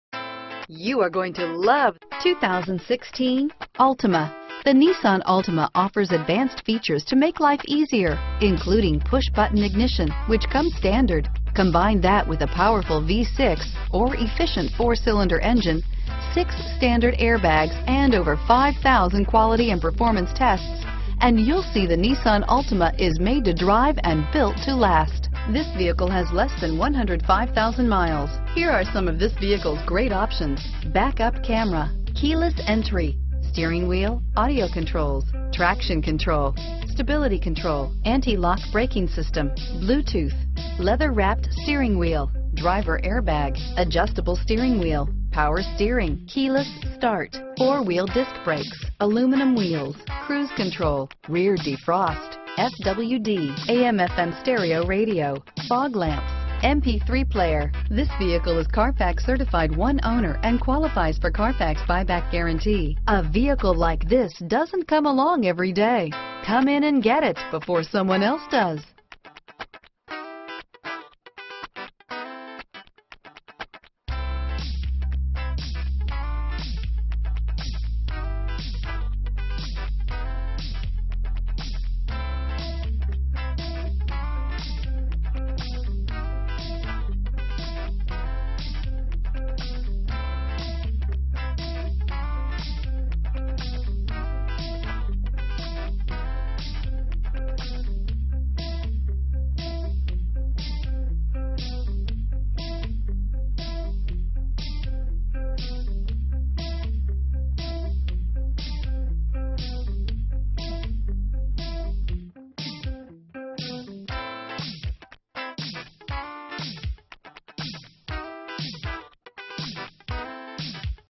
2016 Nissan Altima near me Elmhurst, Carol Stream, Bloomingdale, Itasca, Hinsdale, IL 00447A
Super Black Used 2016 Nissan Altima available near me in Elmhurst, Illinois at Honda on Grand. Servicing the Carol Stream, Bloomingdale, Itasca, Hinsdale, IL area.
2016 Nissan Altima 2.5 SR - Stock#: 00447A - VIN#: 1N4AL3AP2GC268961

2016 Nissan Altima Black CARFAX One-Owner. 2.5 SR FWD CVT with Xtronic 2.5L 4-Cylinder DOHC 16V ABS brakes, Electronic Stability Control, Illuminated entry, Low tire pressure warning, Remote keyless entry, Traction control.Awards:* 2016 KBB.com 10 Best Sedans Under $25,000 27/39 City/Highway MPGCall or Come to Honda On Grand in Elmhurst and get a deal that you can smile about!